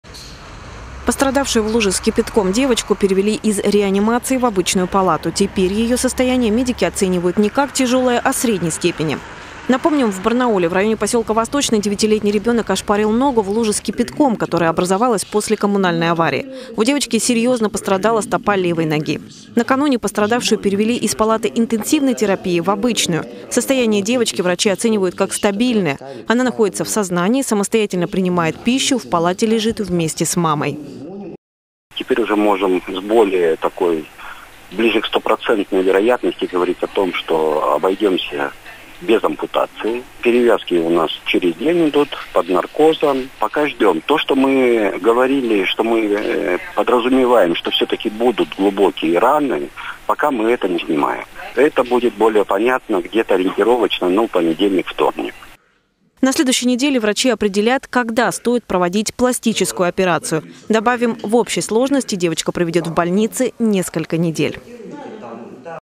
Обварившуюся кипятком девочку перевели из реанимации в обычную палату
Пострадавшую в луже с кипятком девочку перевели из реанимации в обычную палату. Теперь ее состояние медики оценивают не как тяжелое, а средней степени.